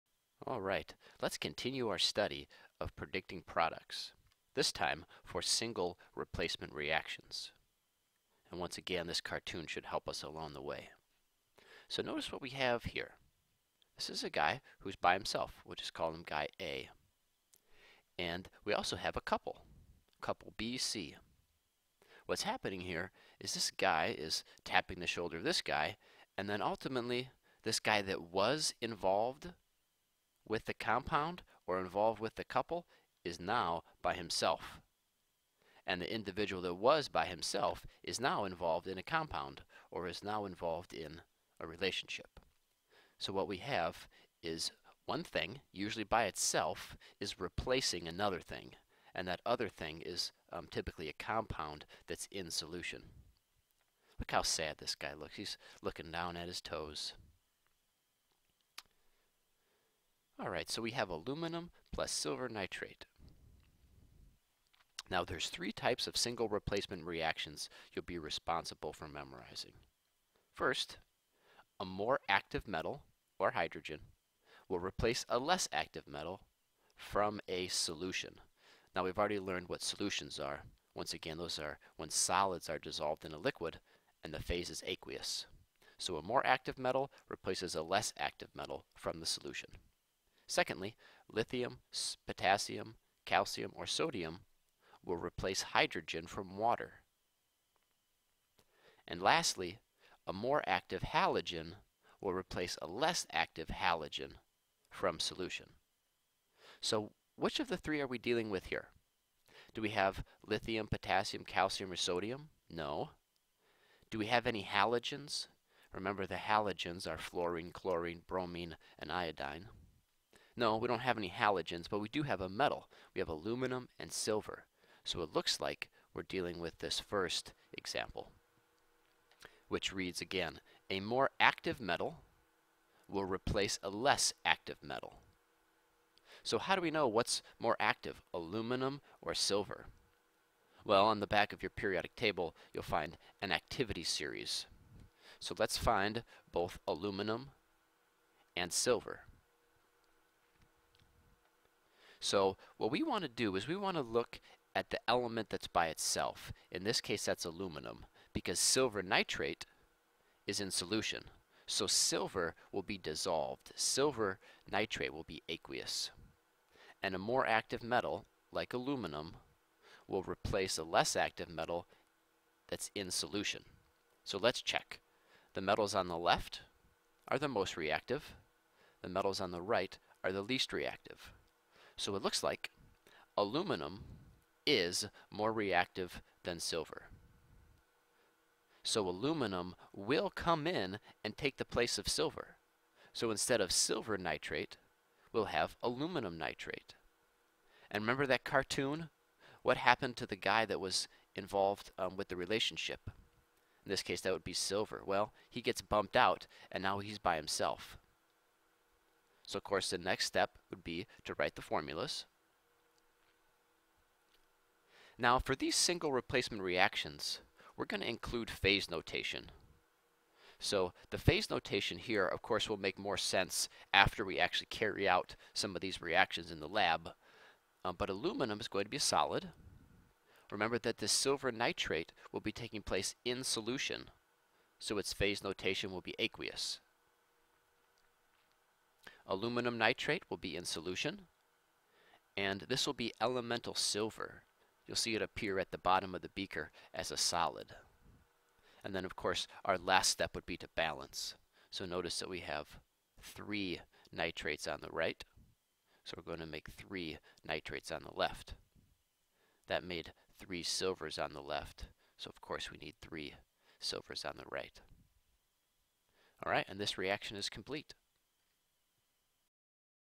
# 8 Predicting Products Single Replacement
This video is an introduction to single replacement reactions